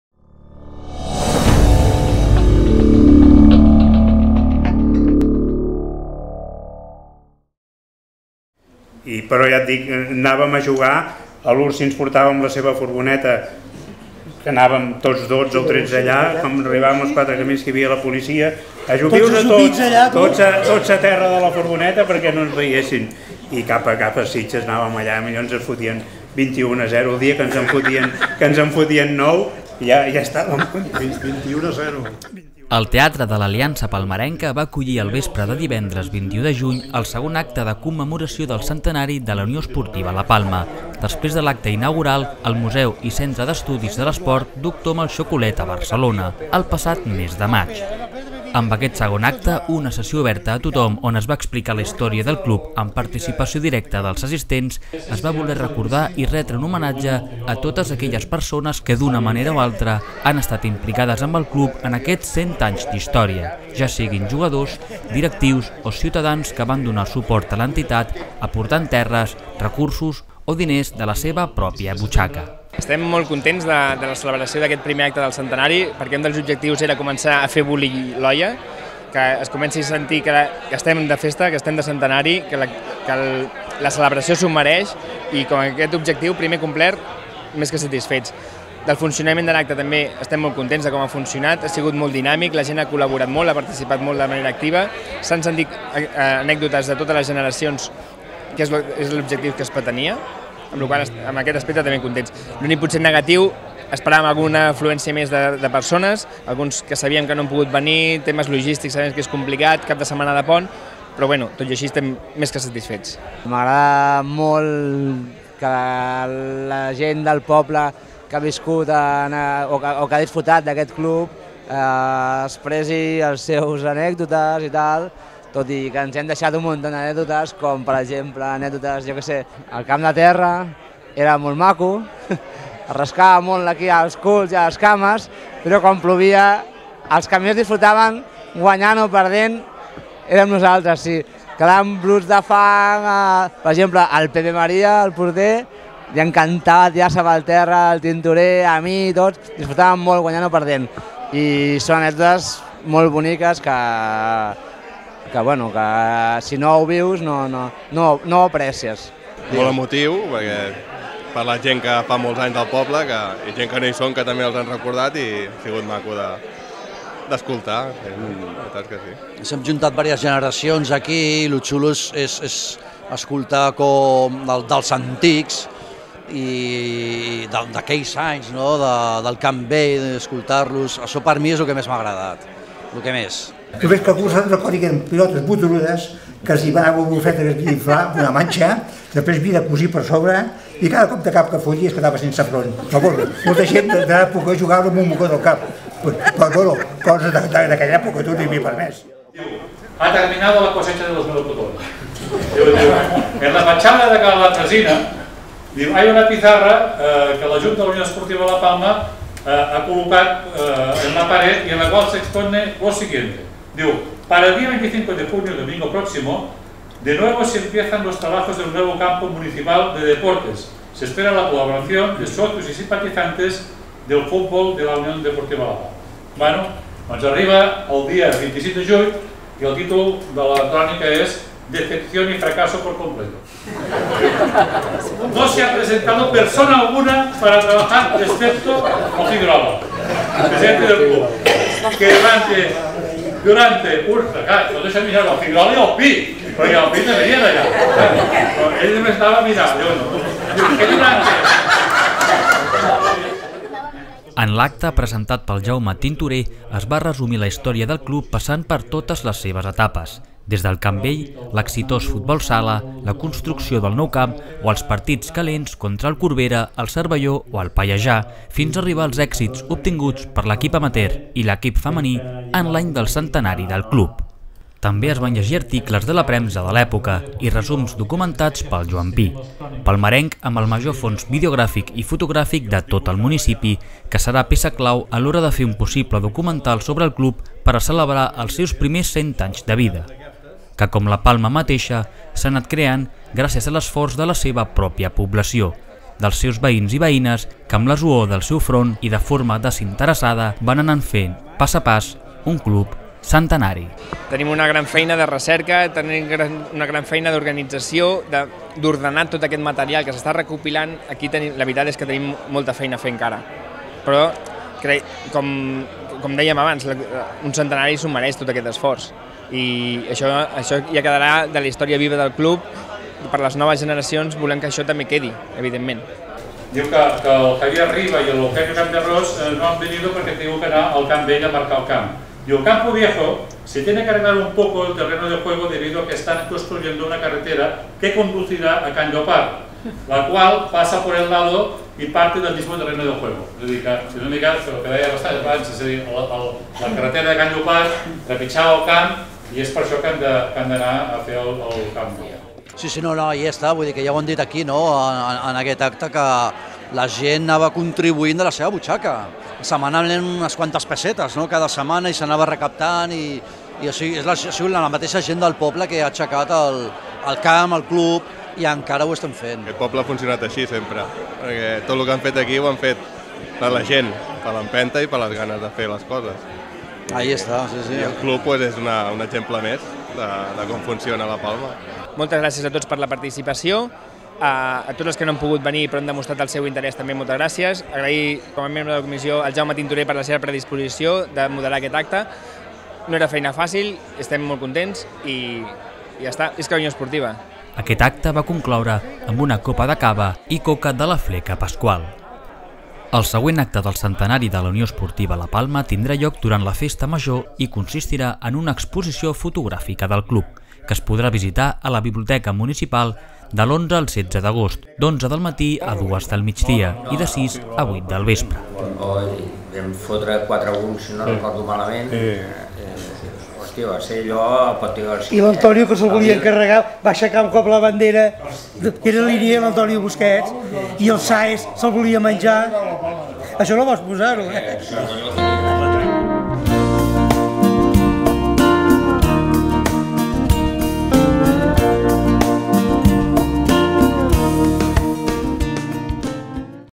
Taula rodona. Centenari de la UE La Palma
Divendres 21 de junt es fa ver una taula rodona de la UE La Palma al teatre de l'Aliança Palmarenca, un dels actes programats per celebrar els 100 anys del club de futbol. L'acte va ser conduit per en Jaume Tintoré i la gent va poder participar explicant anècdotes i curiositats viscudes.